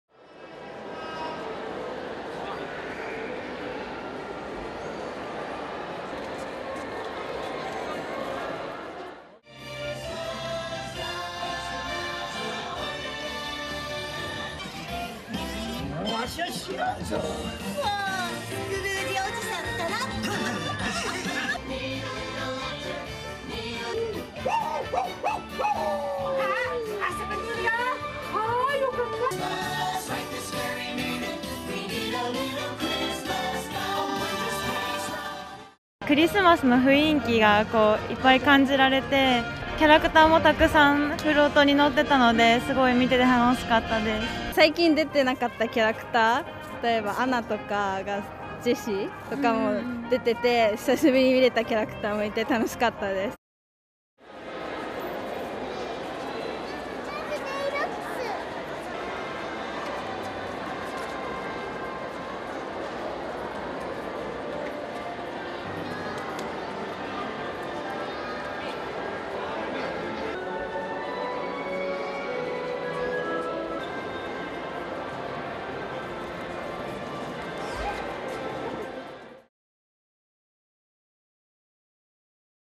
一足先にディズニークリスマス 8日から特別プログラム
千葉県浦安市の東京ディズニーランドと東京ディズニーシーで7日、特別イベント「ディズニー・クリスマス」のプログラムが事前公開された。8日開始で12月25日まで。期間限定のパレードや食事を楽しめる。